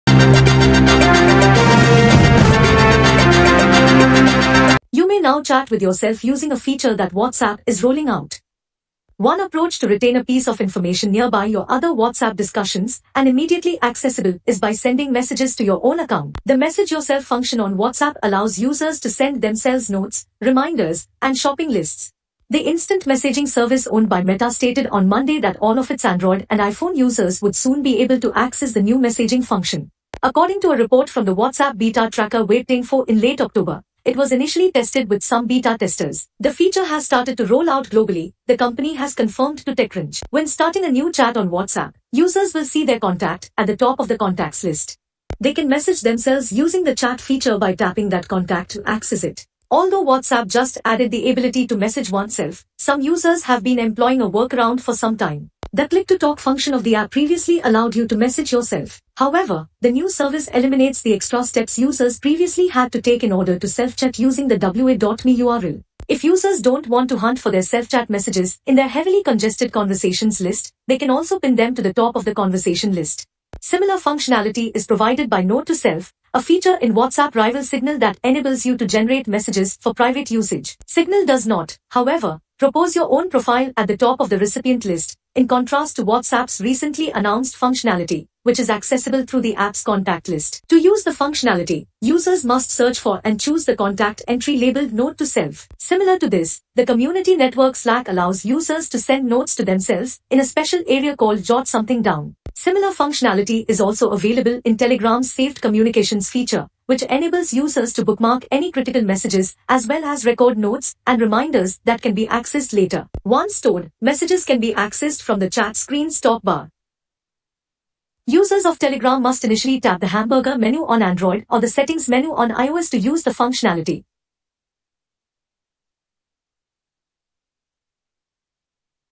WhatsApp begins rolling out 'Message Yourself' feature worldwide
You may now chat with yourself using a feature that WhatsApp is rolling out. One approach to retain a piece of information nearby your other WhatsApp discussions and immediately accessible is by sending messages to your own account.

The "Message Yourself" function on WhatsApp allows users to send themselves notes, reminders, and shopping lists.

The instant messaging service owned by Meta stated on Monday that all of its Android and iPhone users would soon be able to access the new messaging function. According to a report from the WhatsApp beta tracker WABetaInfo in late October, it was initially tested with some beta testers. The feature has started to roll out globally, the company has confirmed to TechCrunch.

When starting a new chat on WhatsApp, users will see their contact at the top of the contacts list. They can message themselves using the chat feature by tapping that contact to access it.

Although WhatsApp just added the ability to message oneself, some users have been employing a workaround for some time. The "click to talk" function of the app previously allowed you to message yourself. However, the new service eliminates the extra steps users previously had to take in order to self-chat using the wa.me URL.

If users don't want to hunt for their self-chat messages in their heavily congested conversations list, they can also pin them to the top of the conversation list.
Similar functionality is provided by Note to Self, a feature in WhatsApp rival Signal that enables you to generate messages for private usage. Signal does not, however, propose your own profile at the top of the recipient list, in contrast to WhatsApp's recently announced functionality, which is accessible through the app's contact list. To use the functionality, users must search for and choose the contact entry labelled "Note to Self."
Similar to this, the community network Slack allows users to send notes to themselves in a special area called "Jot Something Down."
Similar functionality is also available in Telegram's Saved Communications feature, which enables users to bookmark any critical messages as well as record notes and reminders that can be accessed later. Once stored, messages can be accessed from the chat screen's top bar. Users of Telegram must initially tap the hamburger menu on Android or the settings menu on iOS to use the functionality.